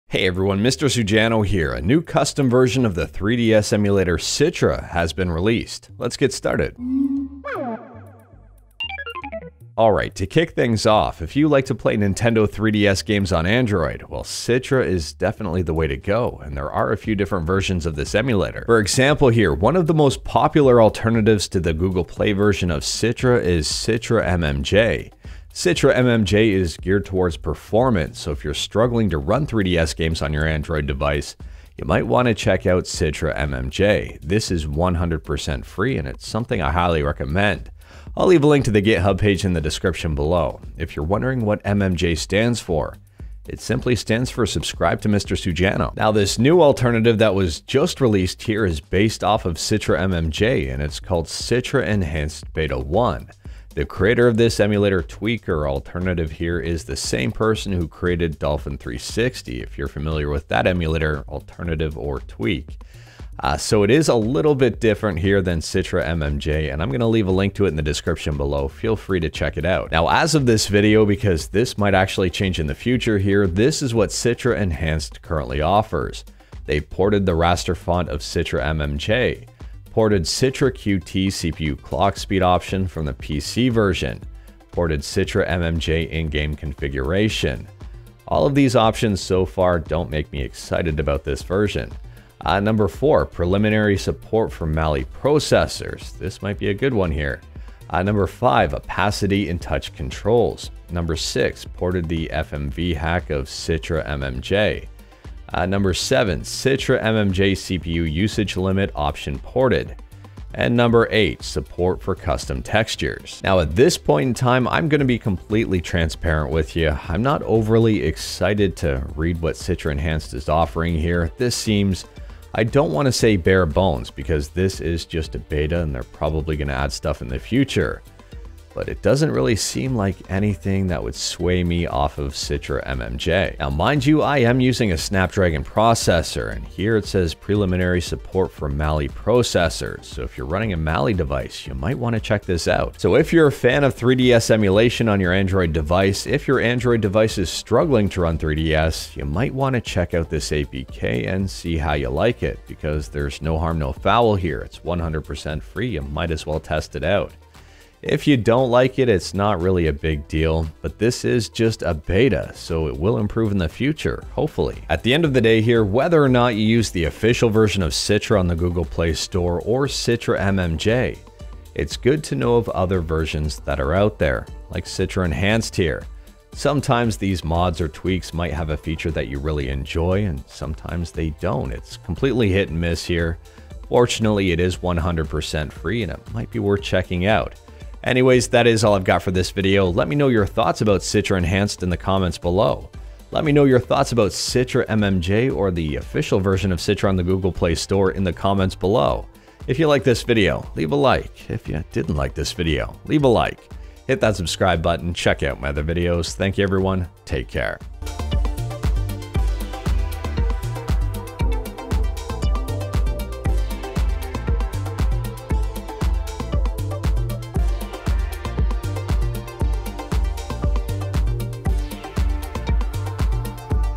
New Citra mod/alternative has been released: 3DS Emulation on Android
Hey All! In this video we go over the new Citra / Citra MMJ Emulator mod that has been released as an APK on android. Citra / Citra MMJ are amazing Nintendo 3DS emulators, let's see what Citra Enhanced has to offer!